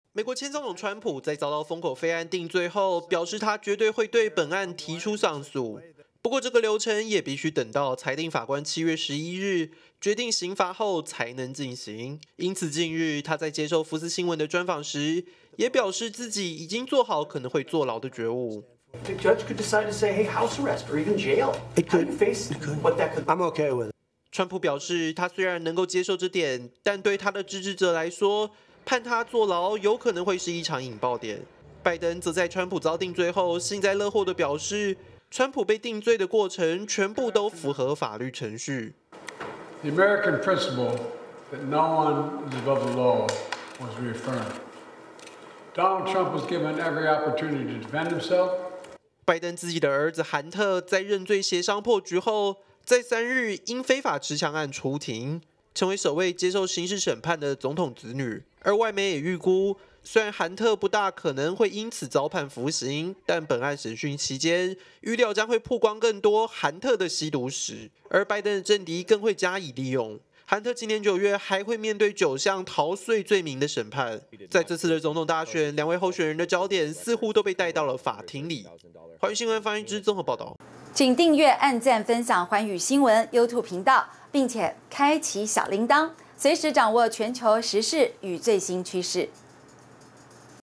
美大選前法庭攻防！川普將上訴.拜登子受審｜寰宇新聞@globalnewstw
美國前總統川普因封口費案遭陪審團認定34項罪名成立，目前正在等待最後的刑罰判決。而川普在接受福斯新聞專訪時表示他已經有了『坐牢的覺悟』，只是他不認為自己的支持者能平靜接受他入獄的結果。另一方面美國總統拜登的兒子韓特，美東時間3日也為『非法持槍案』出庭，成為美國史上第一個首度有『現任總統子女』受審。這屆美國大選似乎成了司法攻防戰。